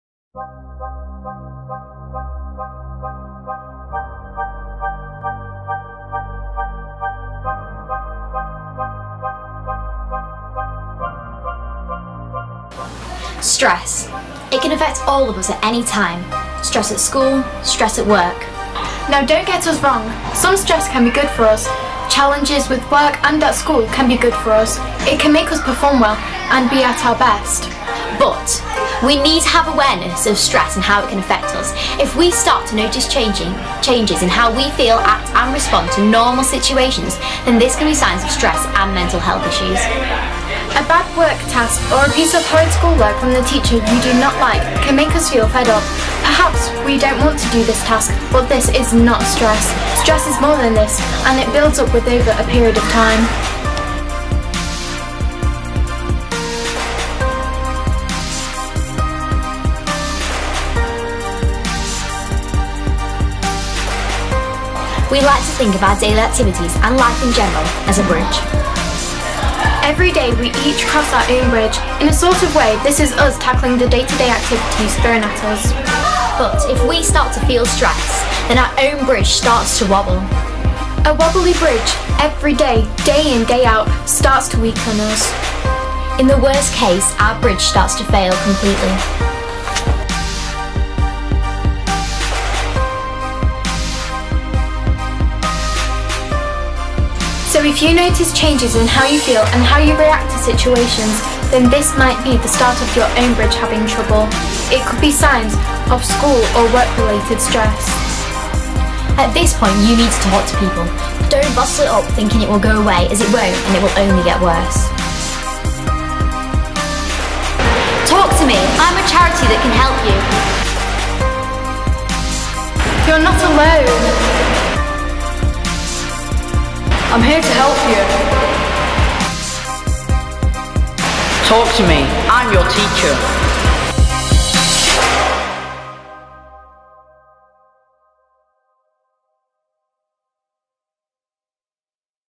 Parklands High School Chorley LOcHERproject MANAGING STRESS
This film by a group of amazing students from Parklands High School in Chorley gives their views on mental health and managing stress. Using a bridge to explain your personal mental health is amazing. When the bridge (your mental health) is solid and stable, every thing is ok. Some days, your bridge will be a bit wobbly, and when you notice this, you should speak to a teacher, colleague or other person. We never want to get to the point where your bridge collapses.